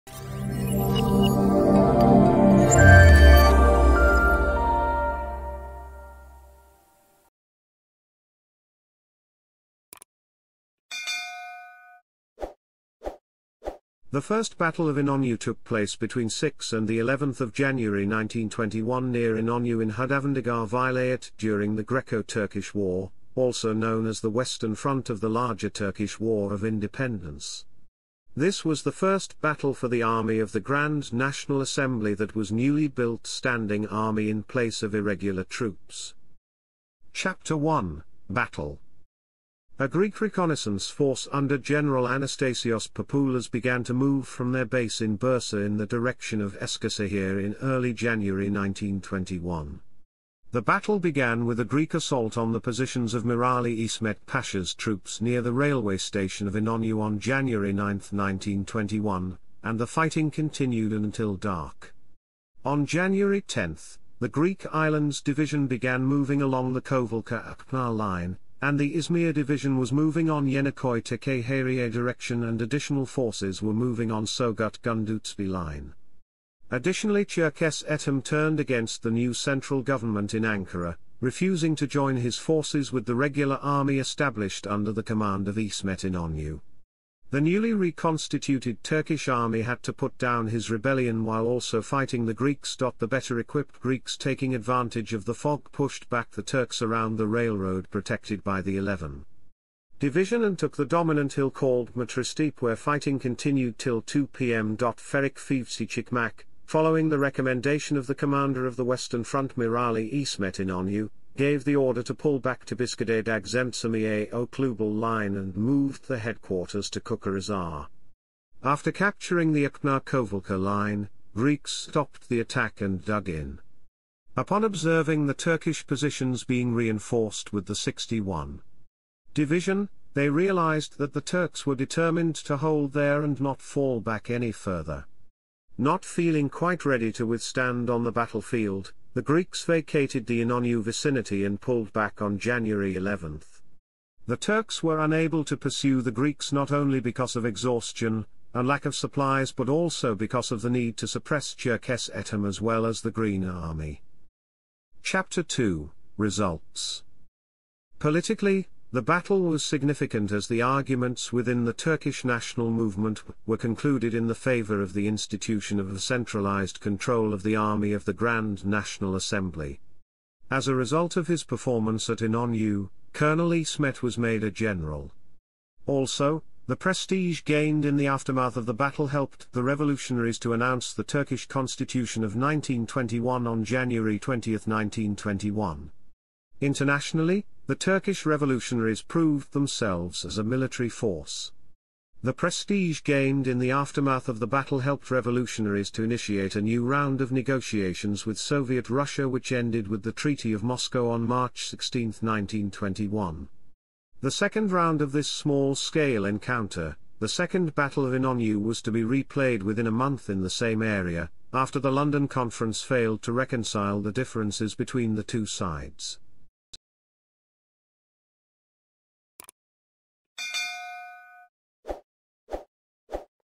First Battle of İnönü - Wikipedia article
Video summary:
The First Battle of İnönü (Turkish: Birinci İnönü Muharebesi) took place between 6 and 11 January 1921 near İnönü in Hüdavendigâr Vilayet (present-day Eskişehir Province, Turkey) during the Greco-Turkish War (1919–22), also known as the western front of the larger Turkish War of Independence. This was the first battle for the Army of the Grand National Assembly that was newly built standing army (Düzenli ordu) in place of irregular troops.